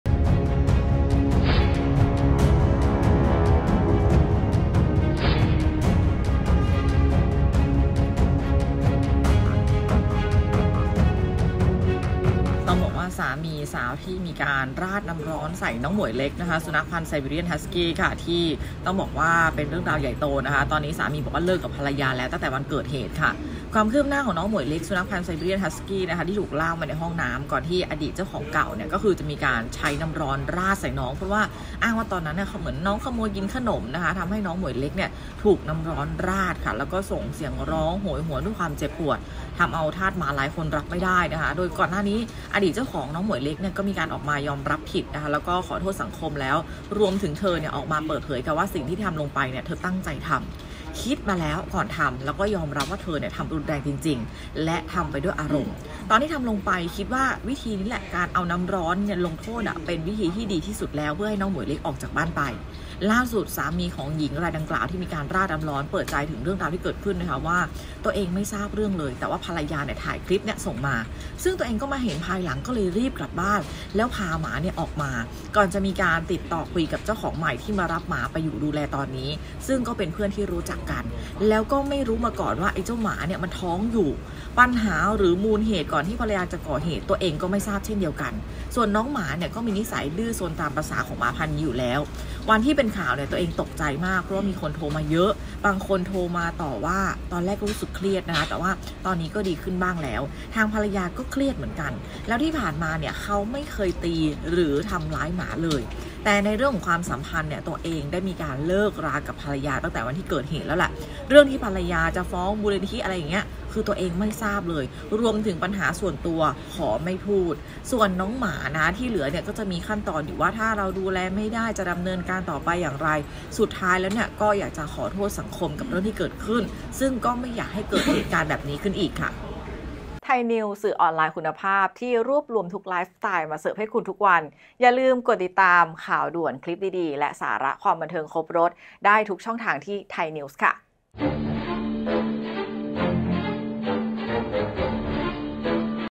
ช็อก! สามีเจ้าของเก่า ‘หมวยเล็ก ไซบีเรียน’ ประกาศเลิกภรรยาแล้ว|Thainews - ไทยนิวส์|Update-16-jj
Update-16-jj

 จากกรณีคลิปไซบีเรียนโดนเจ้าของเดิมลงโทษ ทำให้คนรักหมาต่างหดหู่ใจ ข้อมูลเบื้องต้นพบว่า "หมวยเล็ก" สุนัขพันธุ์ไซบีเรียน ฮัสกี้ เพศเมีย ที่กำลังตั้งท้อง ถูกเจ้าของเดิมทำโทษด้วยความโกรธ ล่าสุดสามีของหญิงรายดังกล่าวทนไม่ไหว ประกาศเลิกภรรยาแล้ว